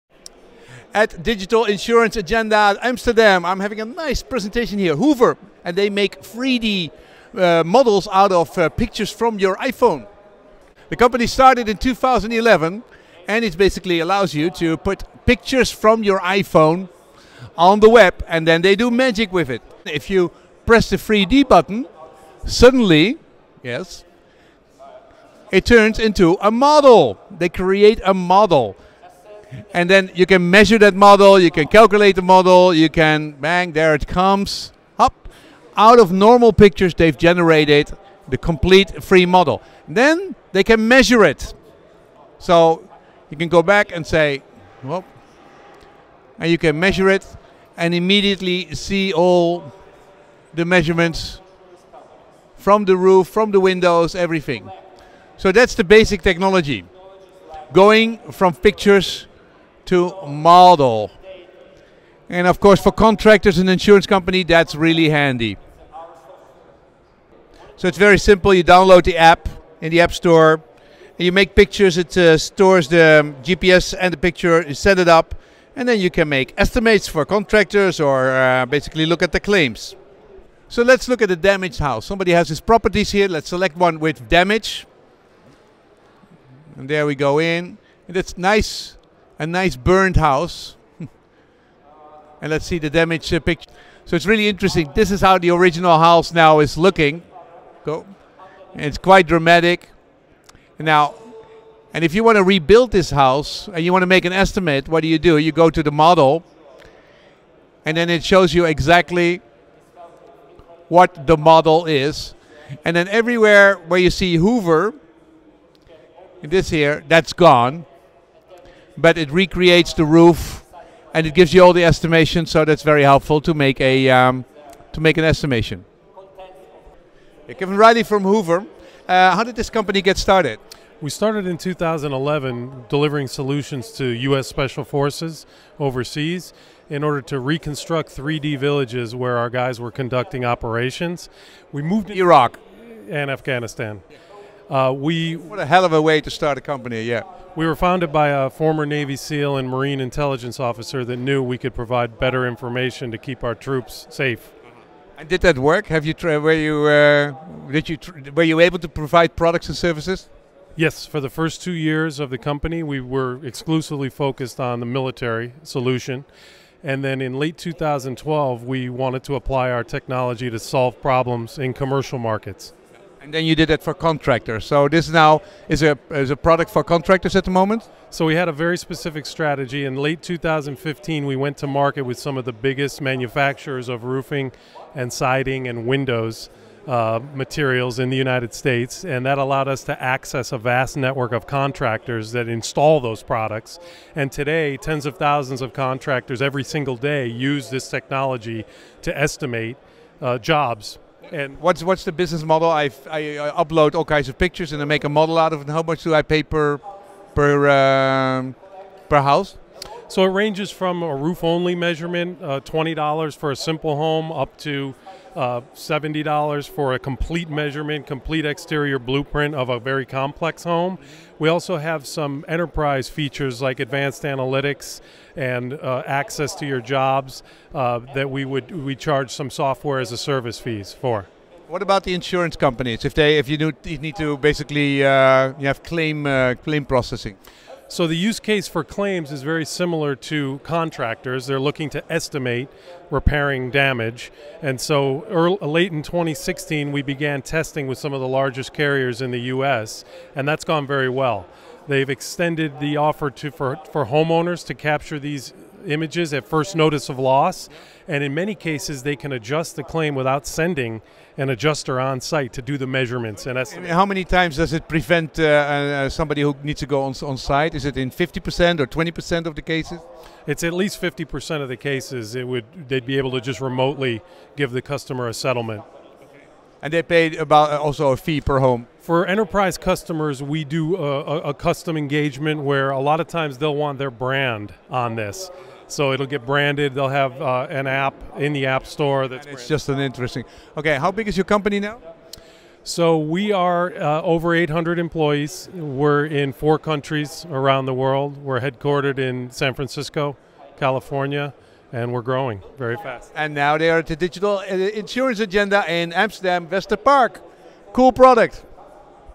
Hover, 3D models of house created by smartphone pictures
Hover makes 3D models of your house by using pictures you took on your phone, an extremely handy feature for contractors and insurance companies! It shows you all measurements, from the roof to the windows to the basement without having to visit the property. The company was founded in 2011 to support USA troops in Iraq/Afghanistan and is now growing very fast having already 800 people working. For 20 dollars you can make a model of a house anywhere in the world!